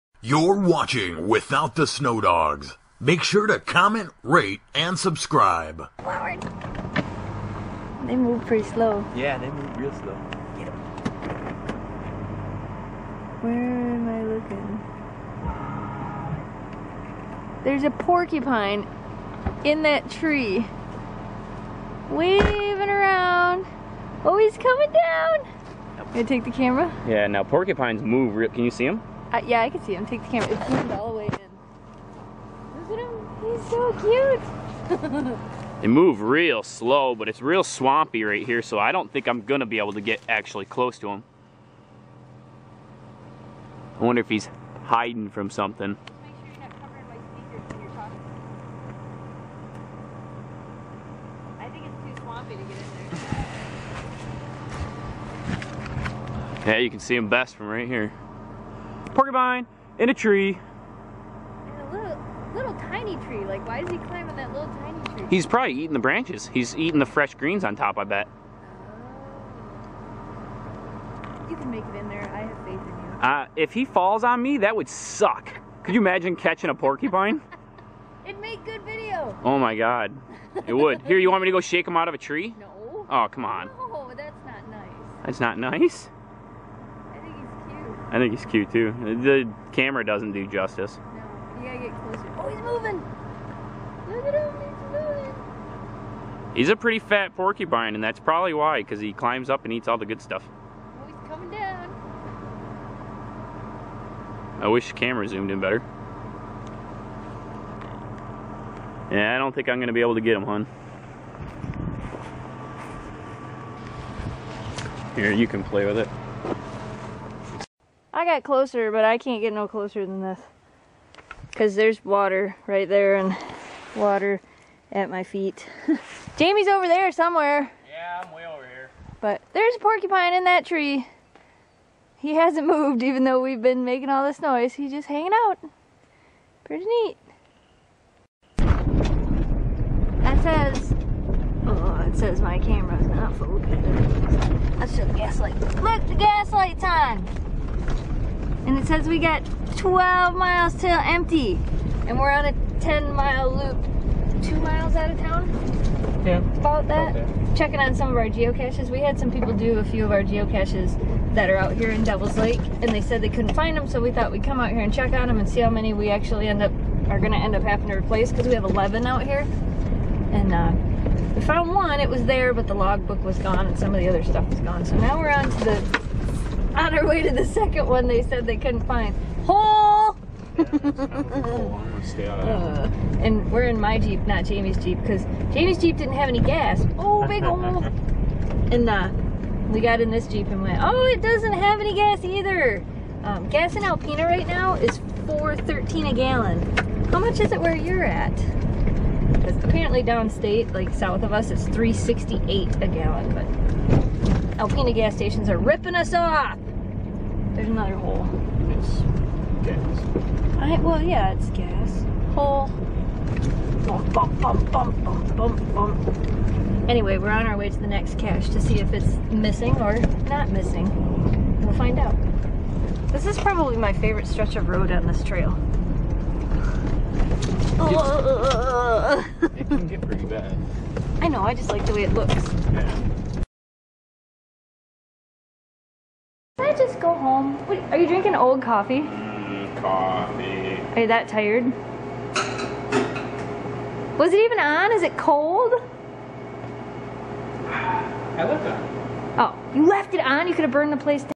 Porcupine up a Tree!!
Went out to check some Geocaches, and found a Porcupine!